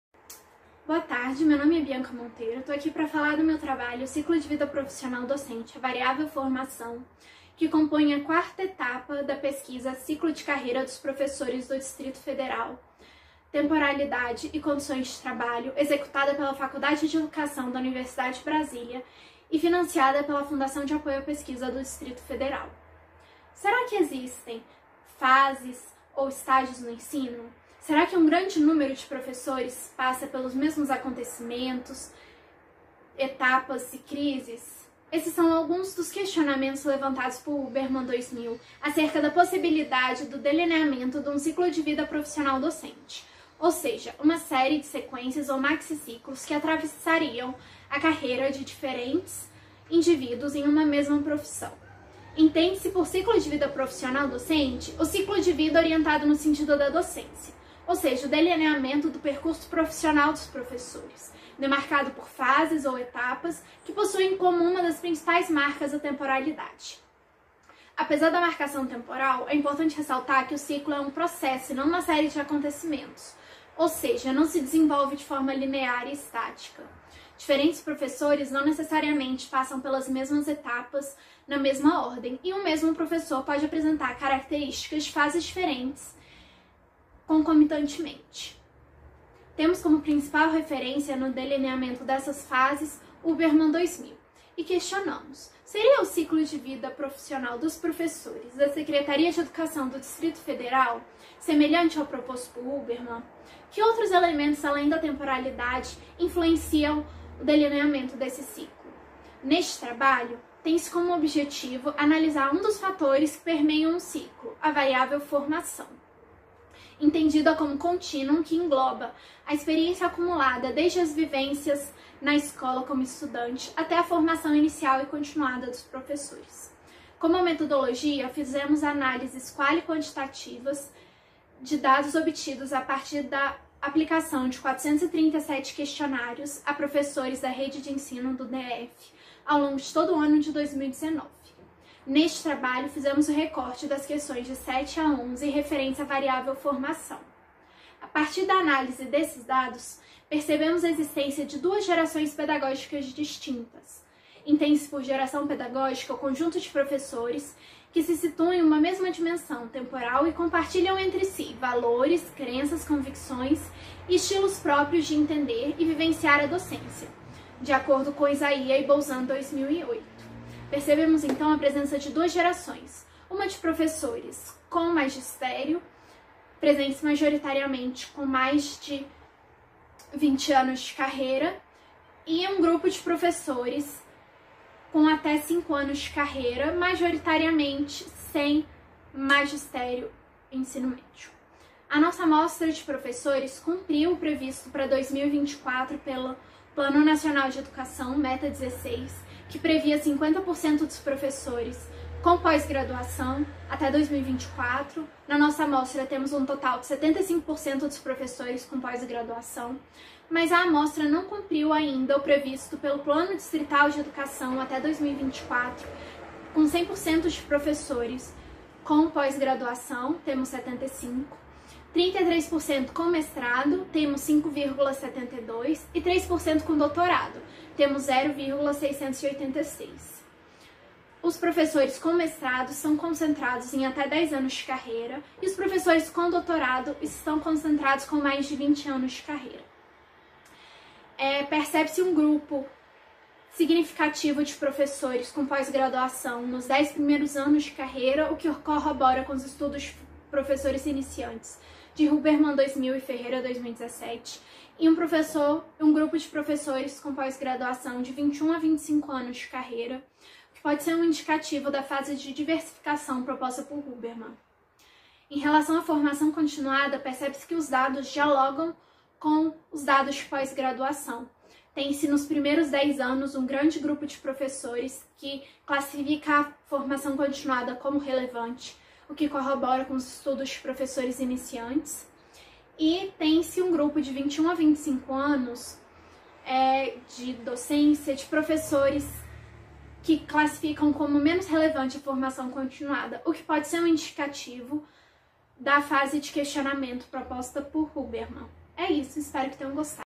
(U2383) O ciclo da carreira docente ao longo do exercício do magistério no contexto do Distrito Fede
"Título: (U2383) O ciclo da carreira docente ao longo do exercício do magistério no contexto do Distrito Federal
Autor: Bianca Resende Monteiro
Orientador: Shirleide Pereira da Silva Cruz
Colaboradores:
Palavras-Chaves: Ciclo de vida profissional docente; Formação; Carreira."